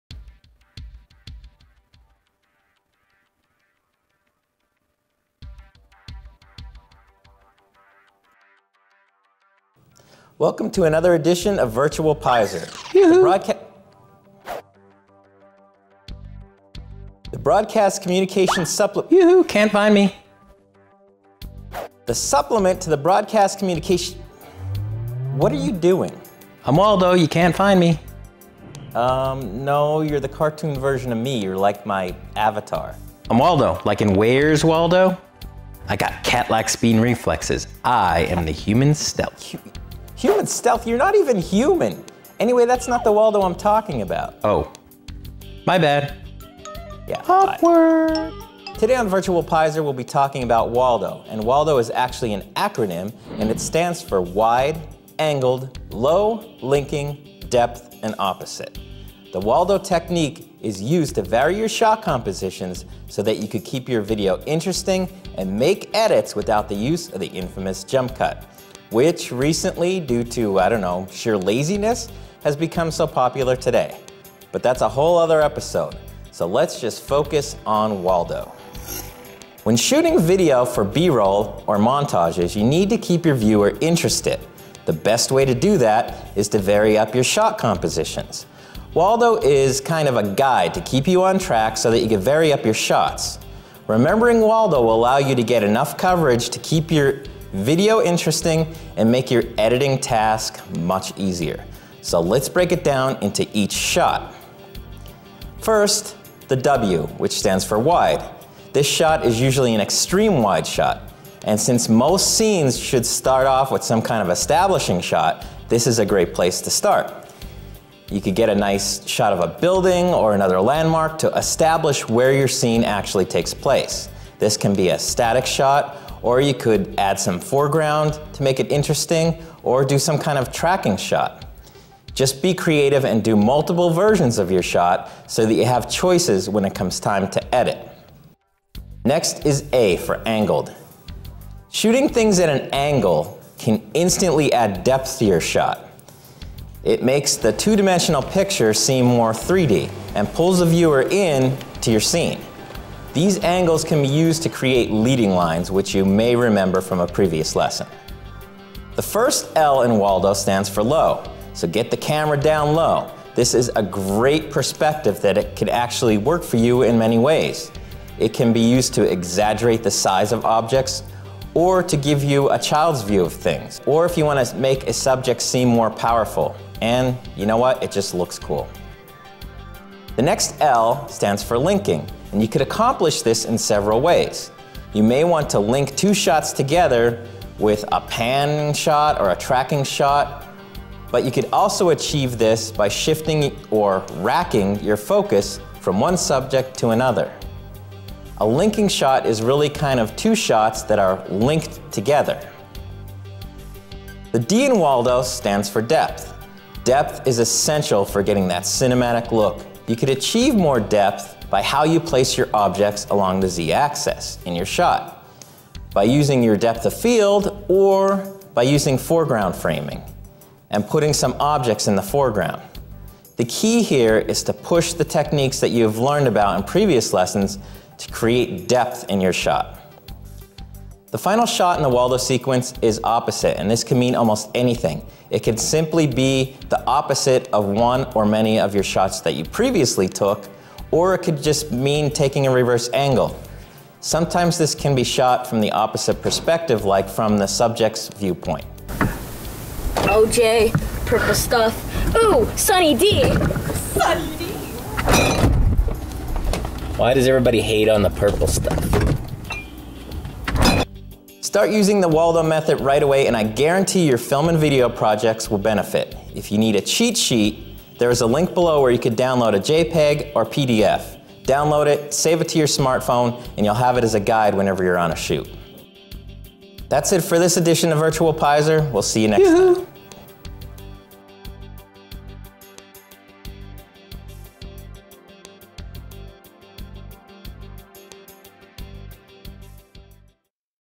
Virtual Peiser Episode #10 WALLDO-Wide, Angled, Low, Linking, Depth, Opposite
Using the WALLDO technique for better film and video.  WALLDO stands for Wide, Angled, Low, Linking, Depth and Opposite and allows you to vary your shot compositions and make your videos more interesting, plus will make your editing task much easier.